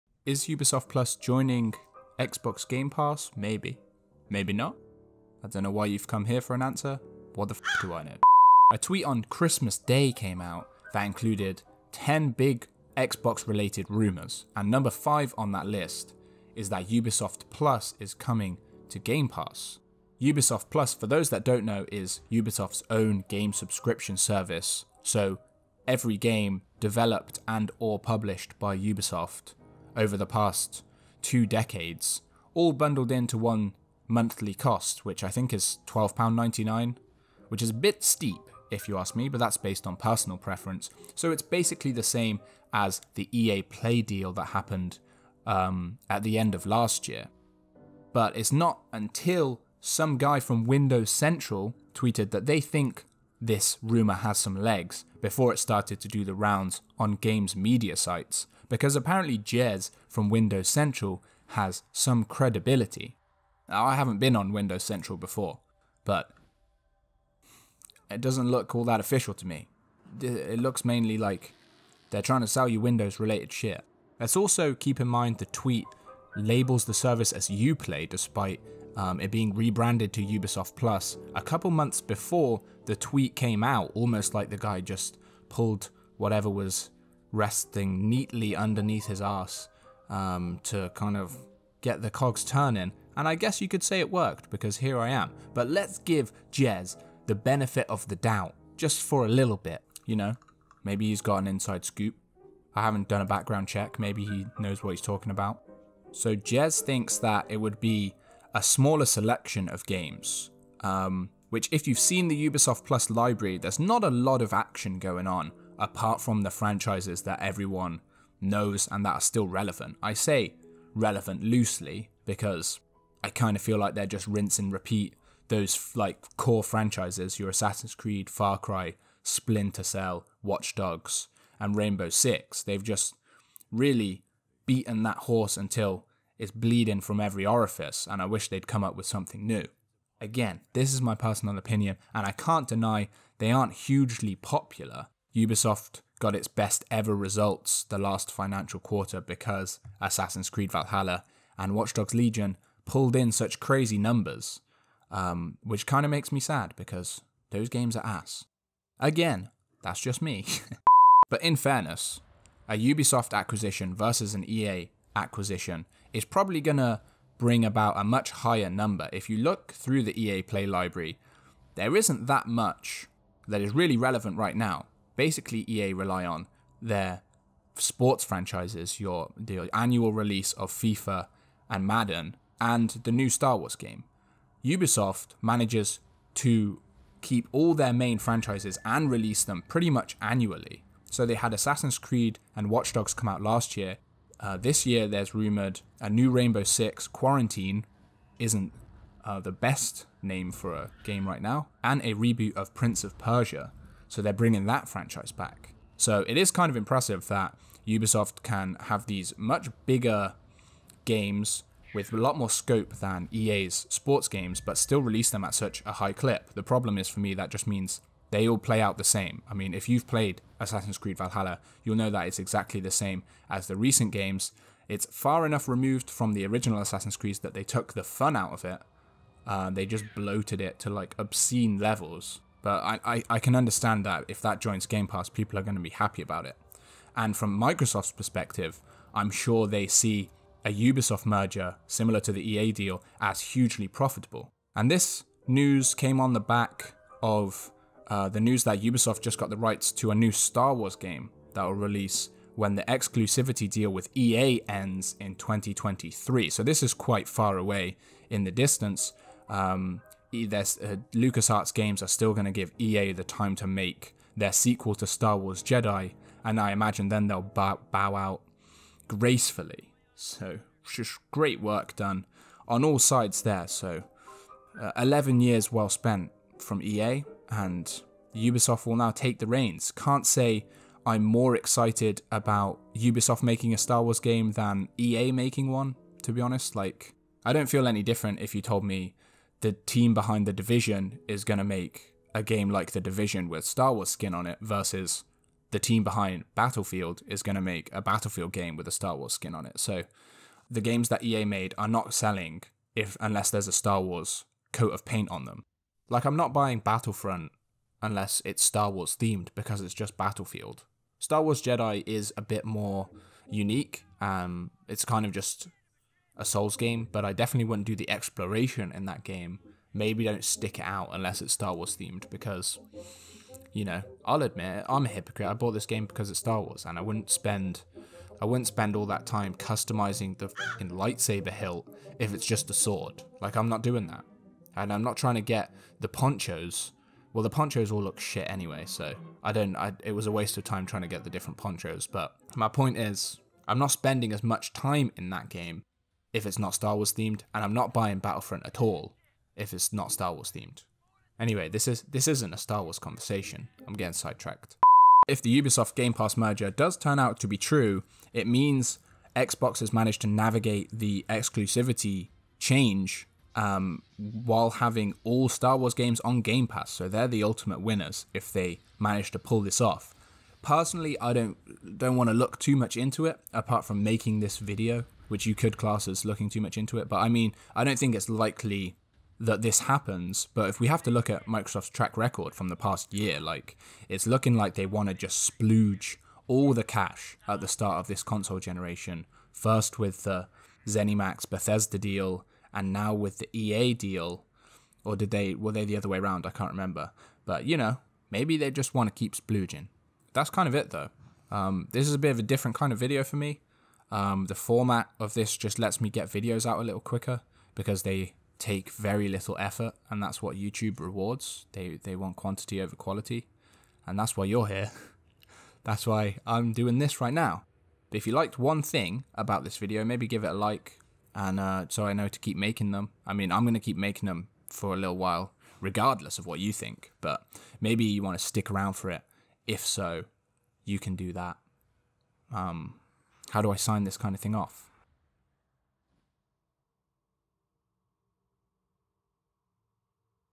Is Ubisoft+ joining Xbox Game Pass?
Xbox Game Pass may be spying a new game subscription merger, this time with Ubisoft. Fresh off the back of its merger with EA Play, rumours are now circling around Ubisoft+ - Ubisoft's publisher-centred subscription for PC users - joining Game Pass. Should you believe such rumours? Your mother taught you better than this.

A Ubisoft+ and Game Pass merger would likely see games like Assassin's Creed Valhalla and Watchdogs Legion join the service, along with series like Tom Clancy, Far Cry, Beyond Good and Evil, and more.

Do you think its likely Ubisoft+ and Game Pass join forces? Would you be excited if they did, even if it meant a price increase? Sound off in the comments.

My channel focuses on game subscriptions Xbox Game Pass, PlayStation Now, EA Play and Ubisoft+.

Title: Is Ubisoft+ joining Game Pass?

0:00 - Ubisoft+ is joining Game Pass gusy
2:40 - What would Ubisoft+ look like on Game Pass
4:05 - Ubisoft strikes deal with LucasFilm Games
5:55 - Ubisoft+ and Xbox Game Pass - is it true tho